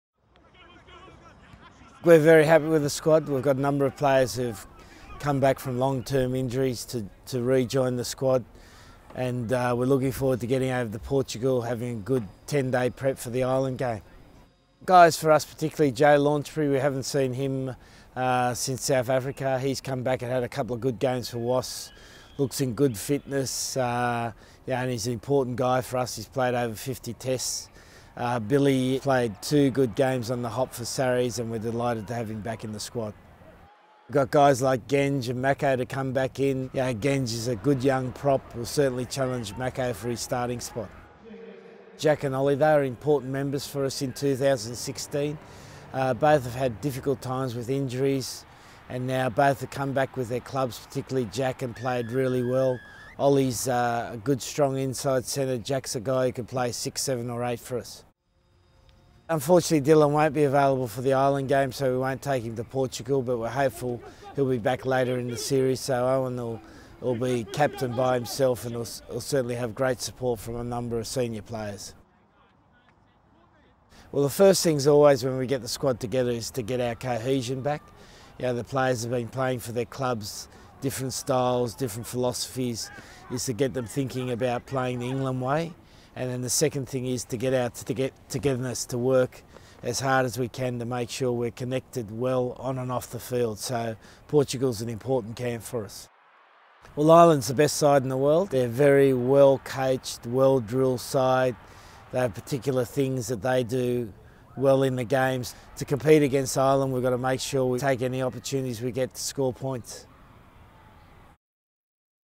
Eddie Jones on his 2019 Guinness Six Nations squad
England men’s head coach Eddie Jones has named a 35-man squad to begin preparations to play Ireland in their opening Guinness Six Nations match in Dublin on 2 February 2019 (kick off 4.45pm, live on ITV).